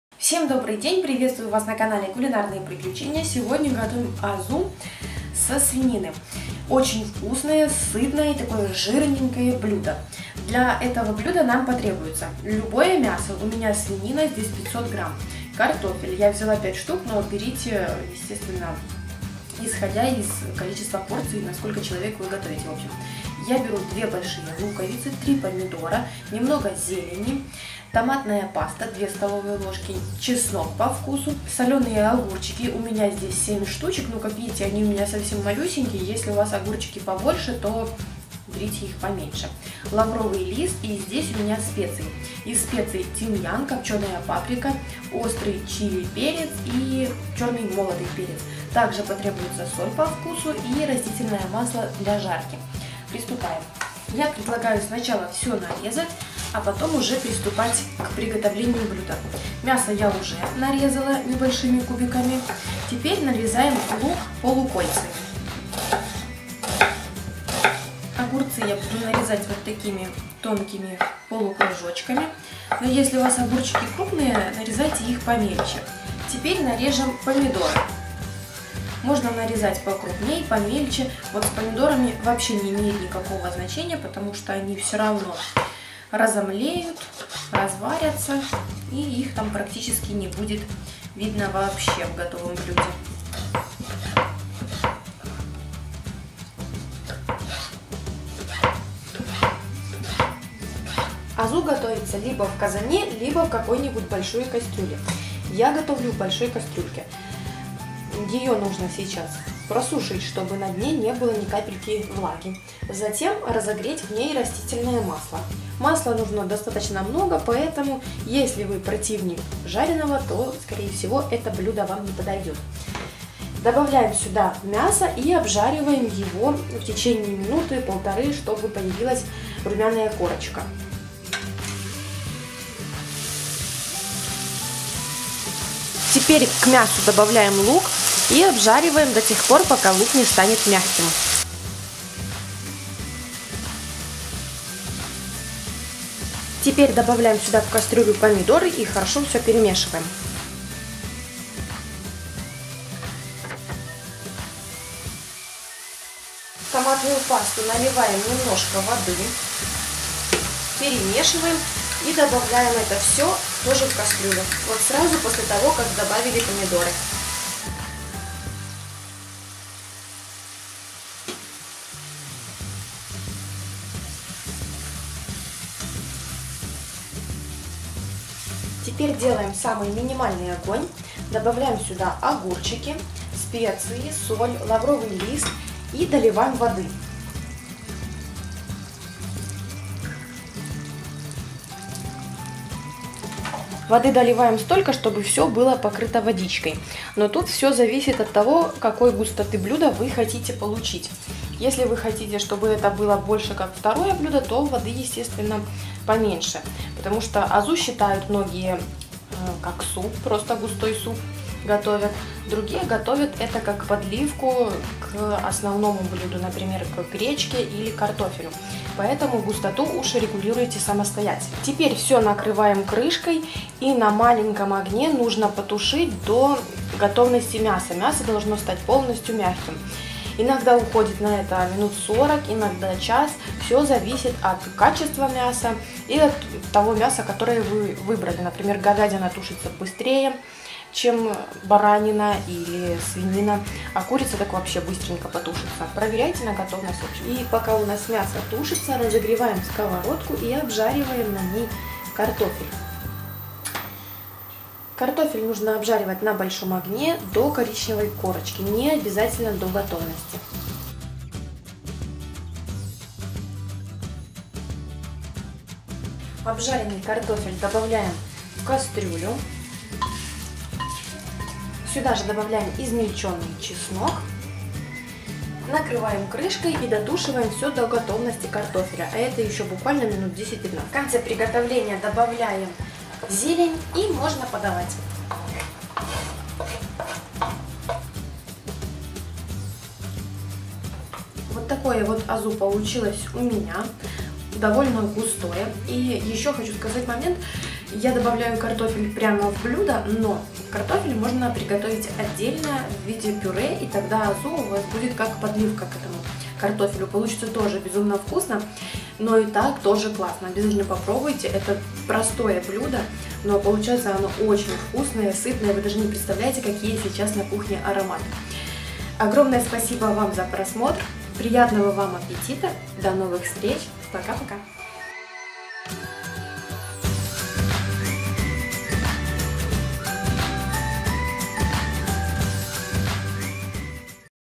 Азу со свининой
Ингредиенты:
- мясо около 500 гр;
- картофель;
- лук;
- огурцы соленые;
- томатная паста;
- помидоры;
- чеснок;
- зелень;
- соль и специи.